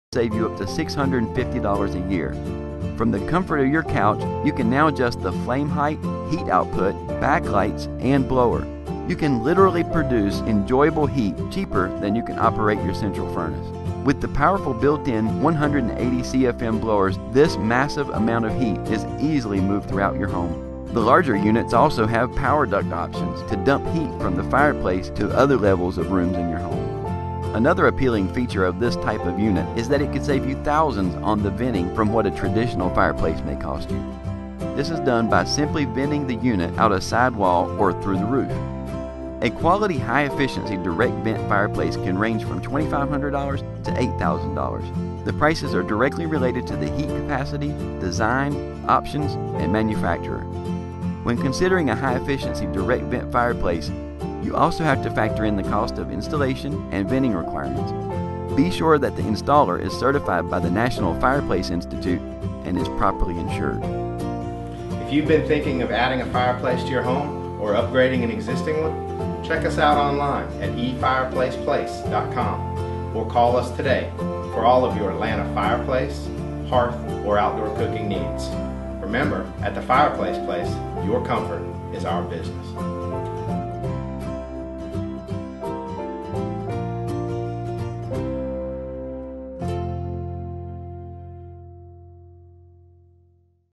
Direct Vent Fireplaces Marietta | The Fireplace Place
Are you ready to get the Direct Vent Fireplace you've always wanted? The Fireplace Place can get you set up in Marietta, and keep it in your price range! Plus our Fireplaces are custom made with each order, and we guide you through each step.

In Store: 1110 Alpharetta St,  Roswell GA 30075